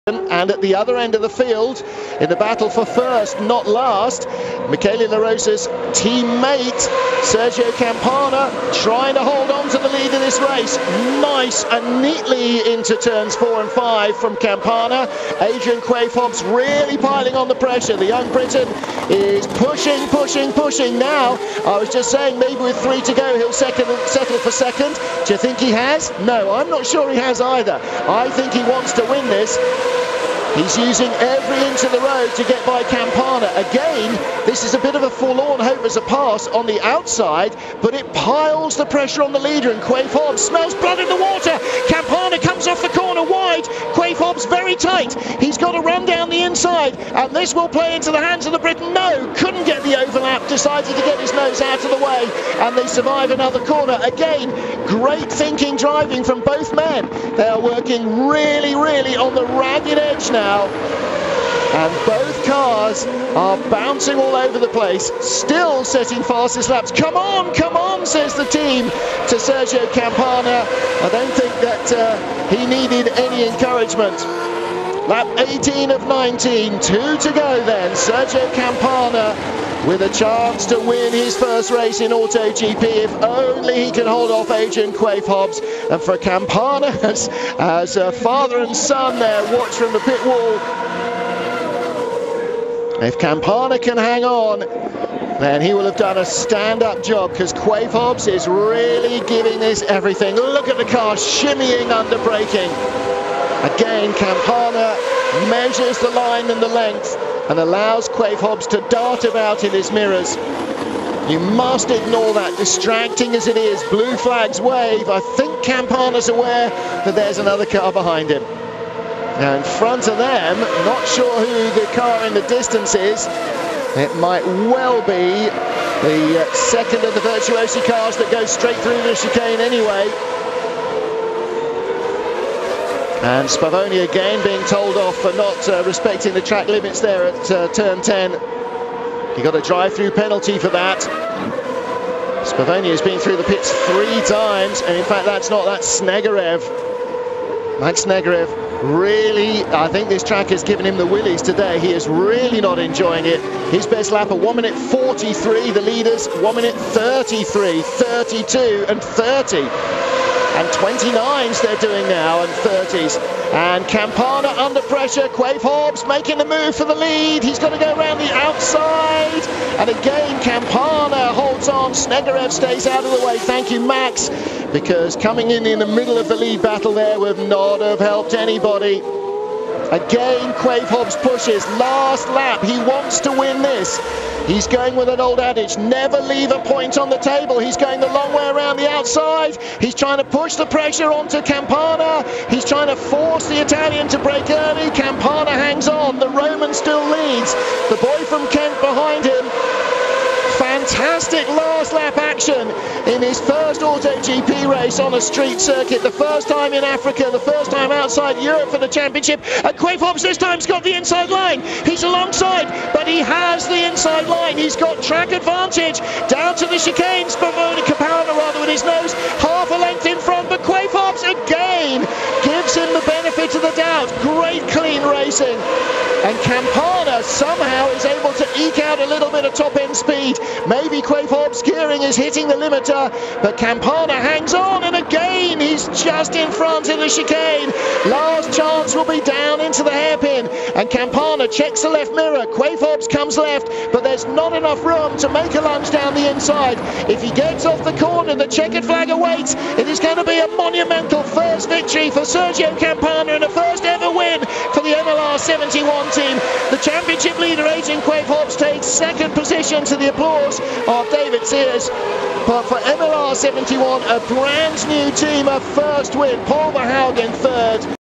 Sergio Campana vs Adrian Quaife-Hobbs, 2012 AutoGP Marrakech
Intense battle between Sergio Campana (MLR71) and Adrian Quaife-Hobbs (Super Nova) in the last laps of the race.
Commentator: Martin Haven